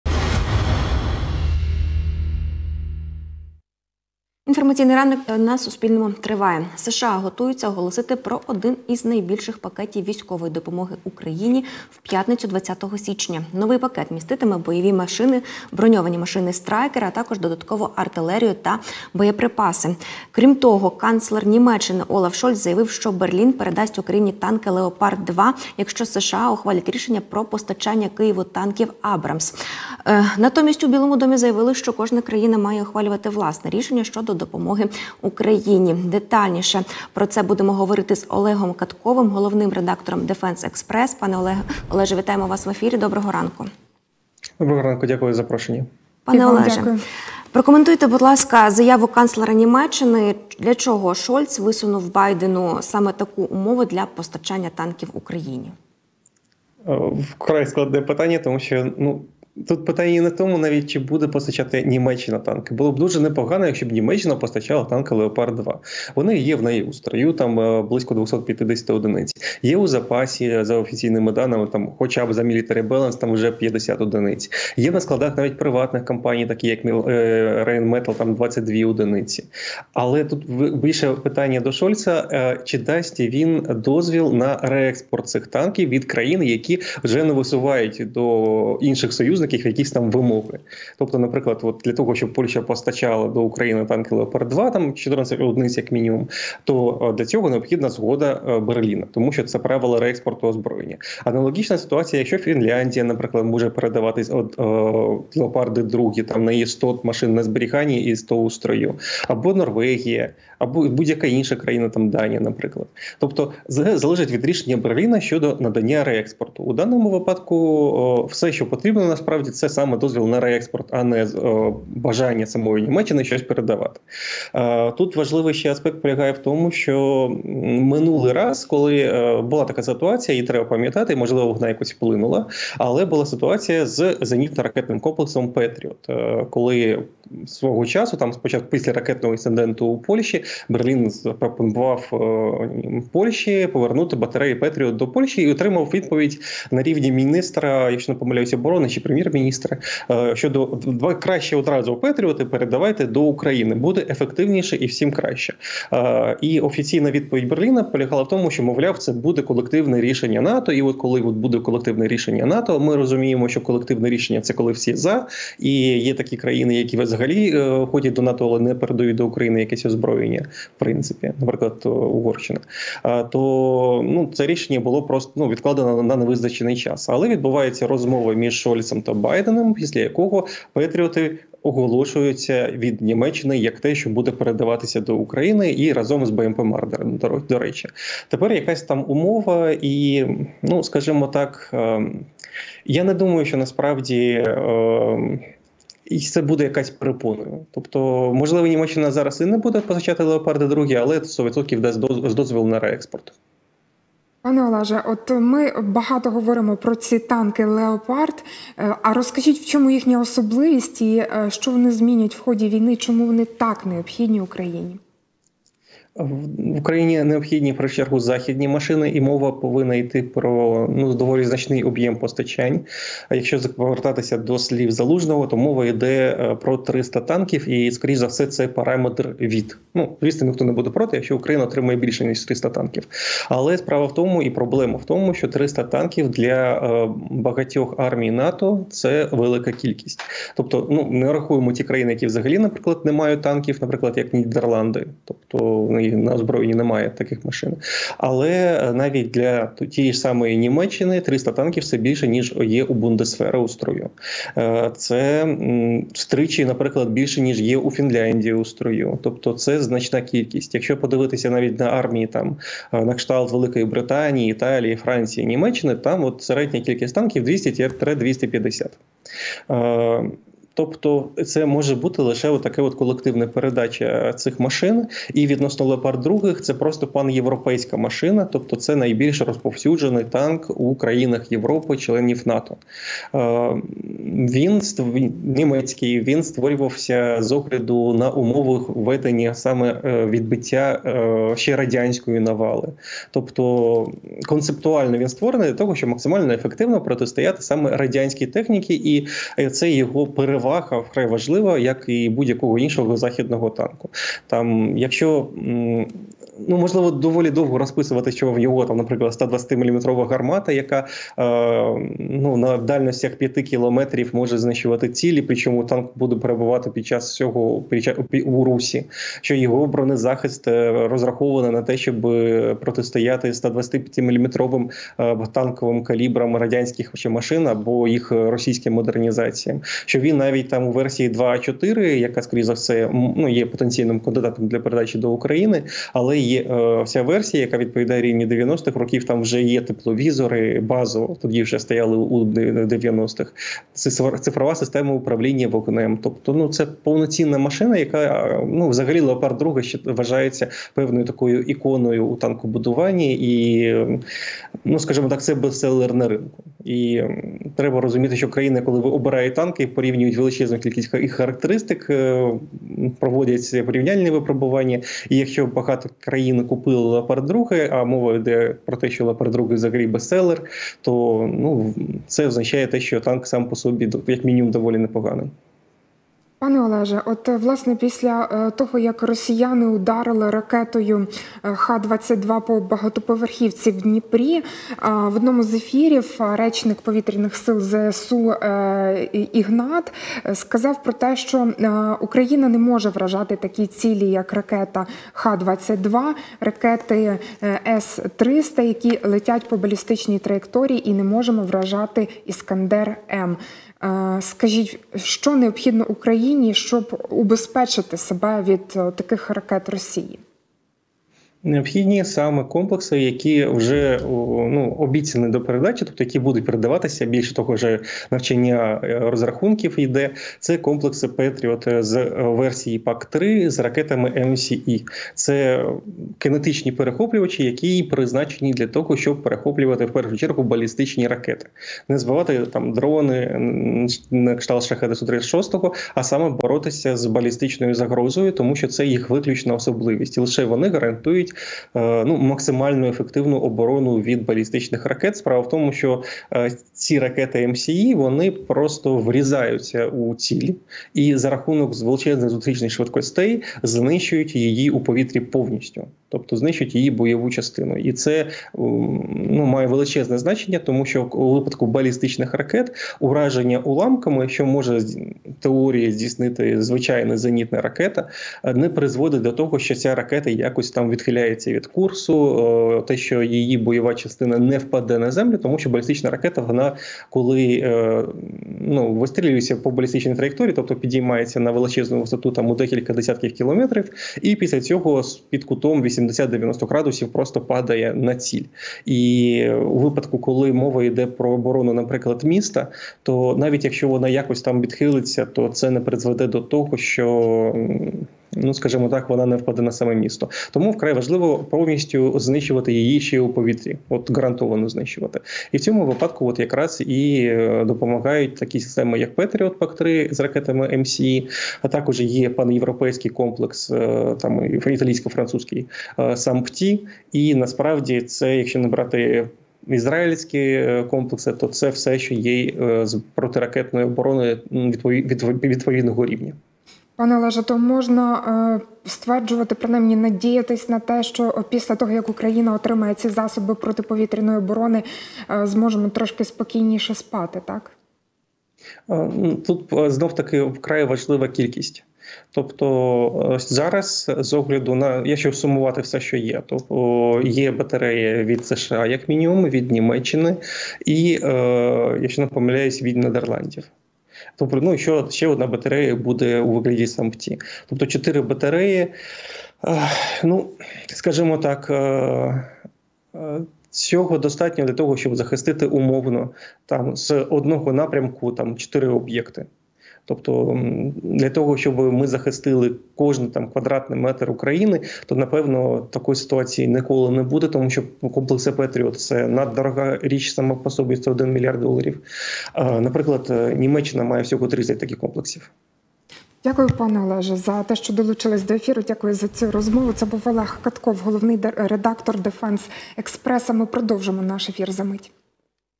Нові надходження військової техніки. Які шанси України отримати Leopard 2 і що потребує військо
США готуються оголосити про один із найбільших пакетів військової допомоги Україні в п’ятницю, 20 січня. Новий пакет міститиме бойові броньовані машини Stryker, а також додаткову артилерію та боєприпаси. Крім того, канцлер Німеччини Олаф Шольц заявив, що Берлін передасть Україні танки Leopard 2, якщо США ухвалять рішення про постачання Києву танків Abrams. Натомість у Білому домі заявили, що кожна країна має ухвалювати власне рішення щодо допомоги Україні.

Головний редактор Defense Express Олег Катков прокоментував заяву канцлера Німеччини та чому українському війську вкрай потрібні західні машини.